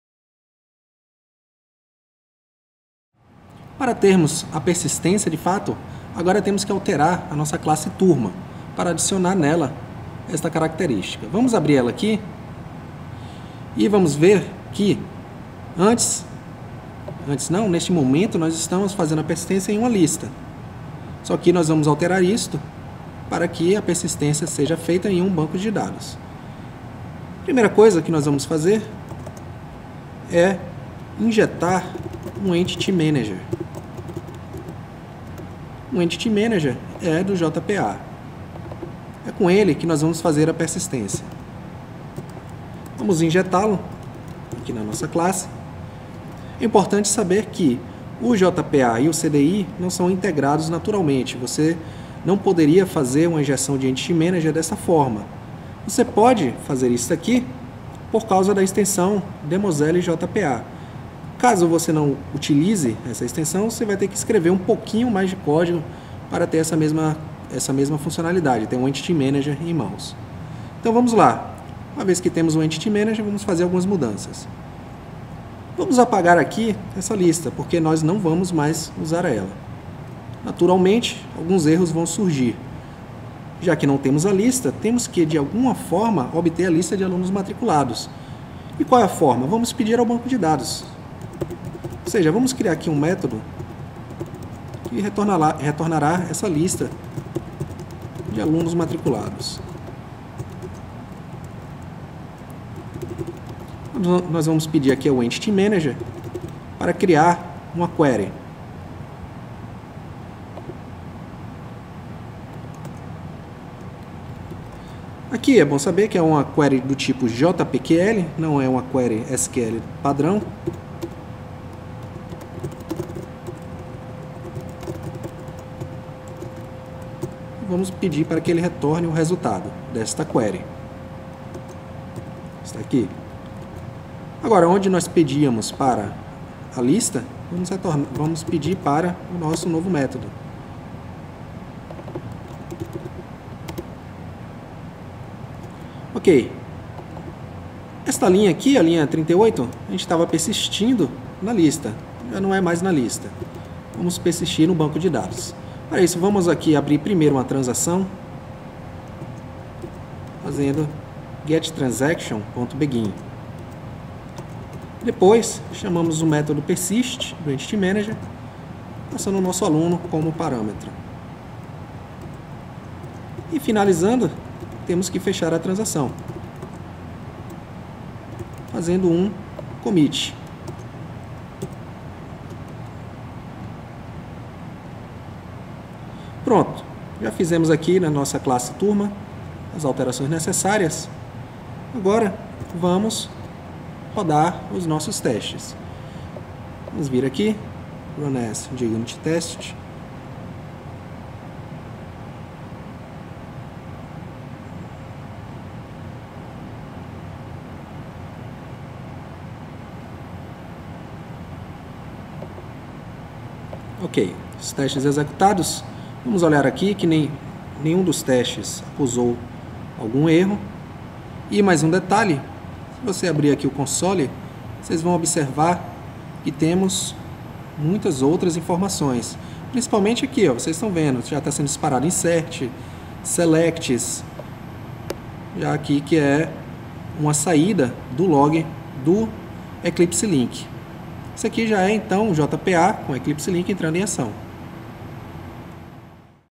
Demoiselle E@D: Módulo 4 - Vídeo 3
Vamos fazer o código necessário para realizar a persistência dos dados usando o EntityManager.